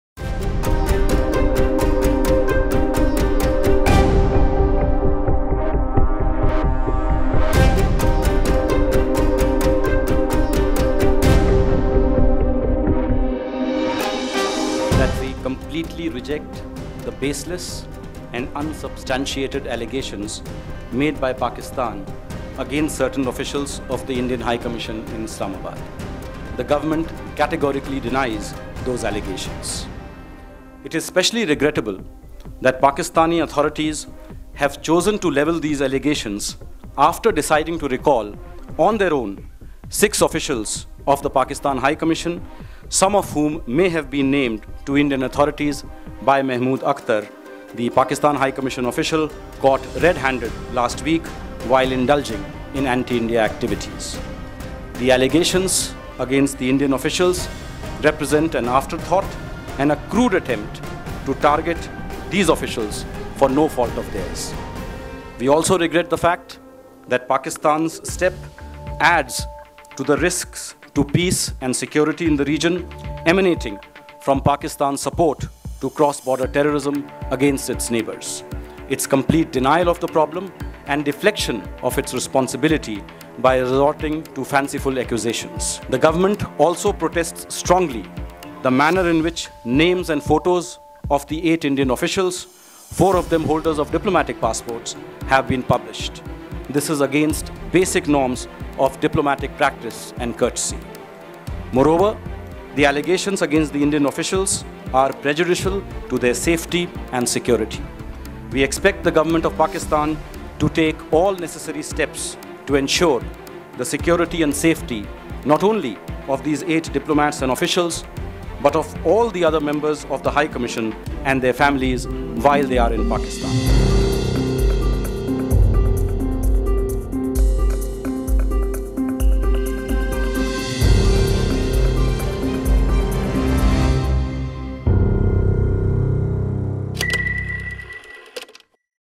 The Quint: Indo-Pak Ties Hit New Low over ‘Tit for Tat’ Spying Allegations
India-Pakistan relations tumbled to a new low as Islamabad on Thursday named eight Indian High Commission officials for alleged spying, which India rejected as "baseless and unsubstantiated" and as part of the neighbours's "tit for tat" move after a Pakistani official was caught "red-handed" for spying.